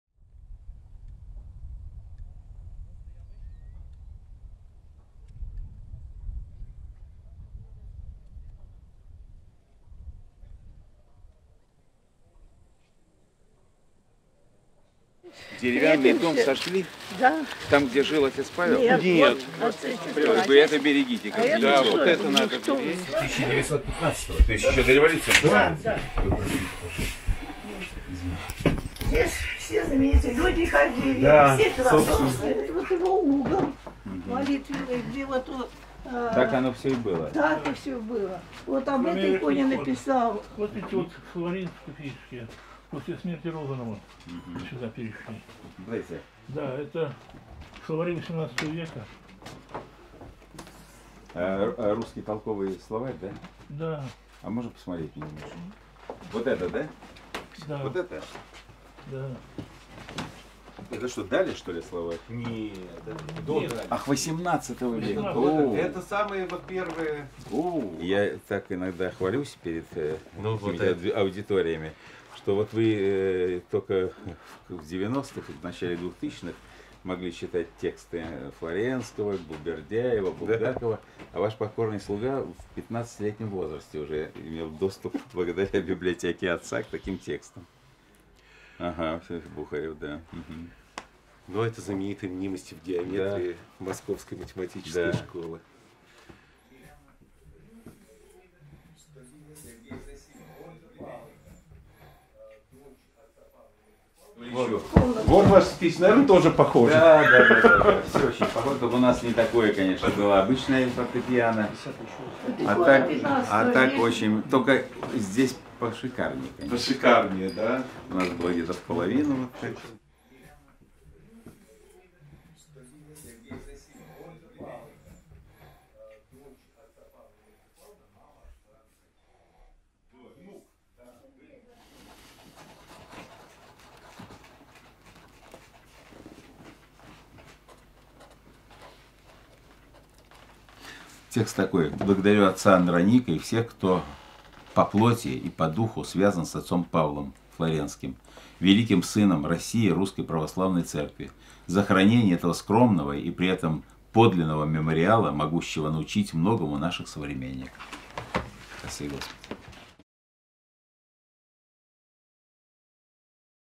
Святейший Патриарх посетил музей-квартиру священника Павла Флоренского
18 июля в подмосковном Сергиевом Посаде Святейший Патриарх Московский и всея Руси Кирилл возглавил церемонию открытия мемориального комплекса «Всем пострадавшим за Христа в годы гонений и репрессий». По окончании церемонии Святейший Патриарх в сопровождении епископа Сергиево-Посадского Парамона и игумена Андроника (Трубачева) осмотрел музей-квартиру священника Павла Флоренского.